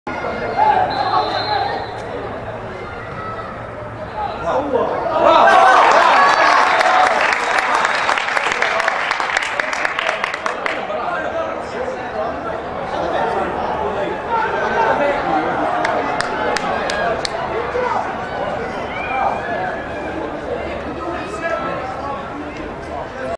هدف النصر الثاني على سدوس برأسية حسن الراهب وصناعة حسين عبدالغني || مباراة ودية
الحساب الرسمي لنادي النصر السعودي